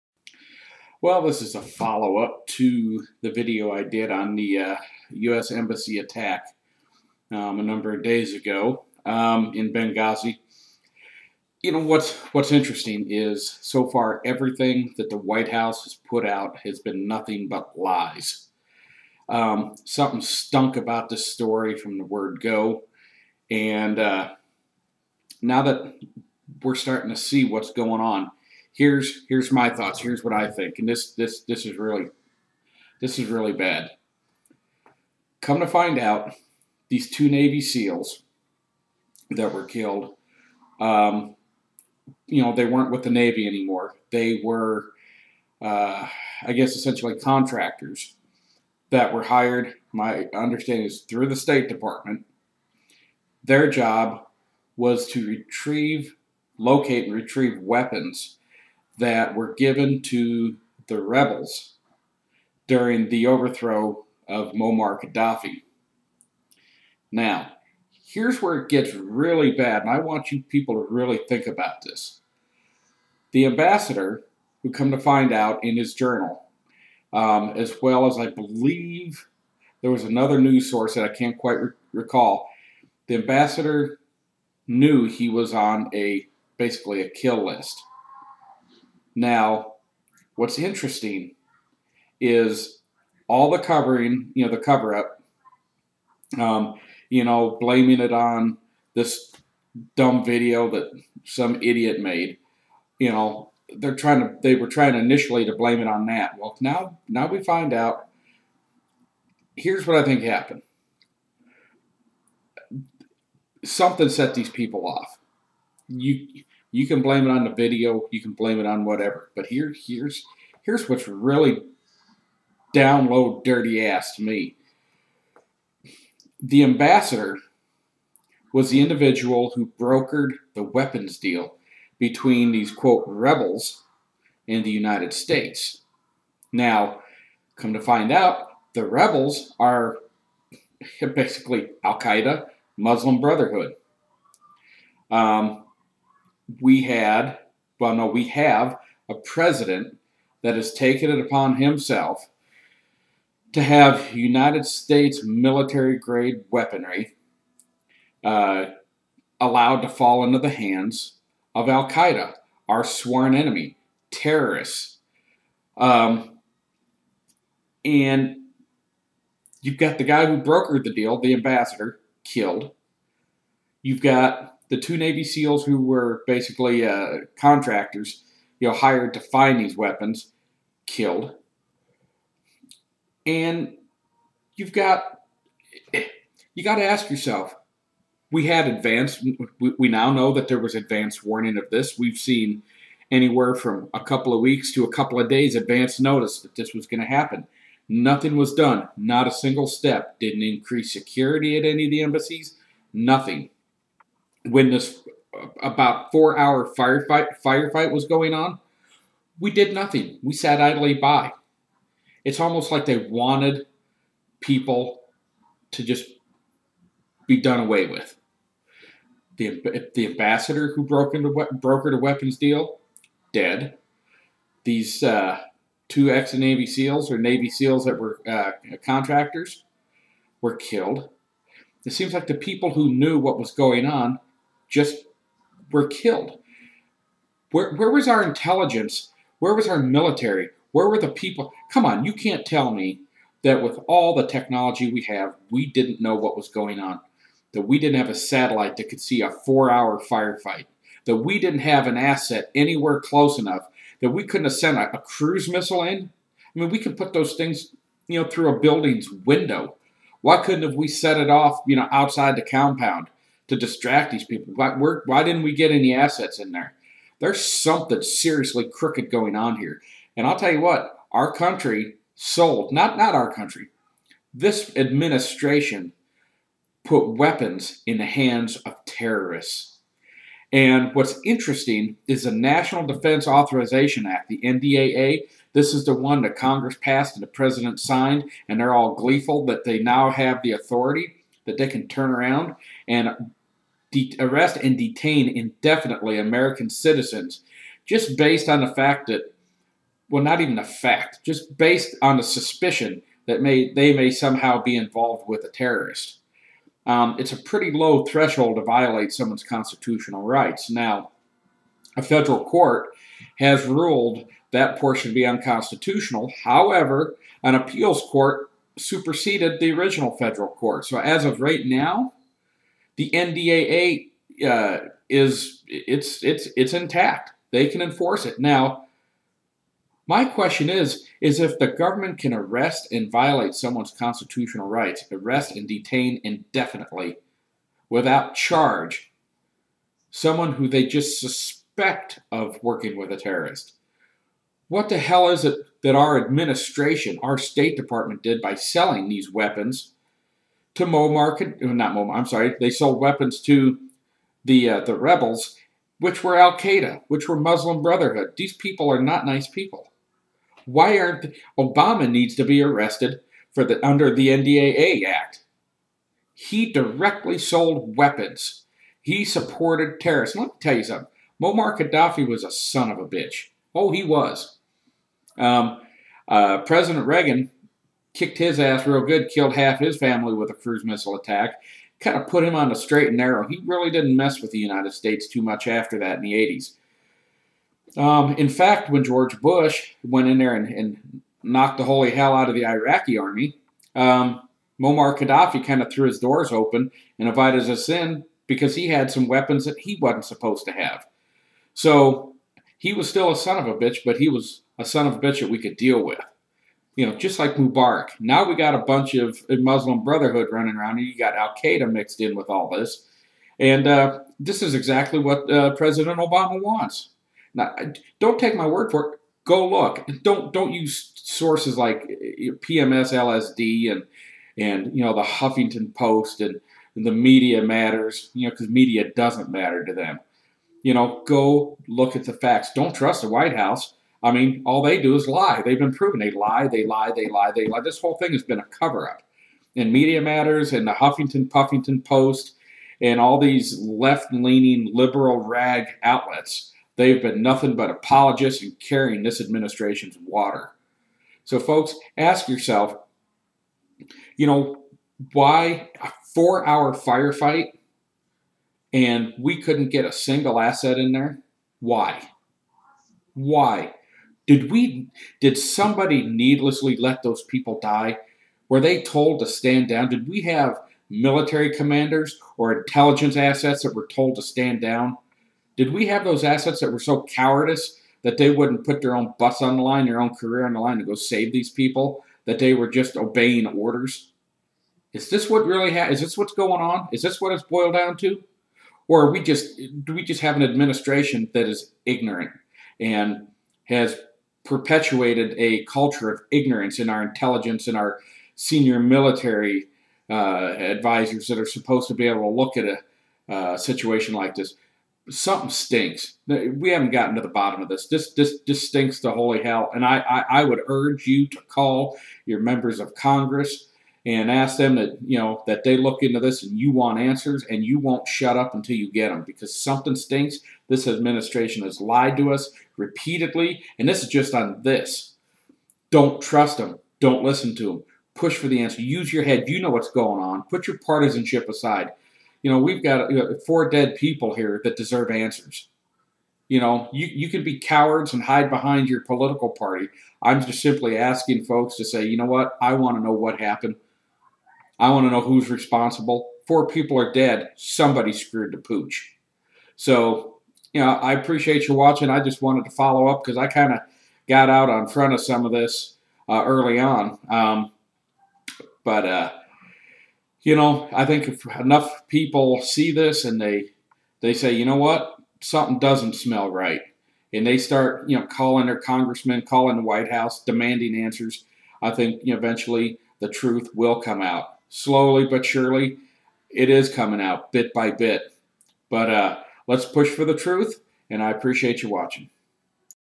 Embassy attack pt2
Something really stinks about this whole cover-up. A 4 hour fire fight we did nothing about, 2 Navy Seals who were looking for weapons the US/Obama Administration sold to the "rebels" that ended up with our enemy Al Queda, the parties that knew of this arms deal are killed, Obama Admin lied and lied blaming a ridiculous video as the cause of the attack. As a citizen I am asking for folks to call your Congressional members and demand they get the truth!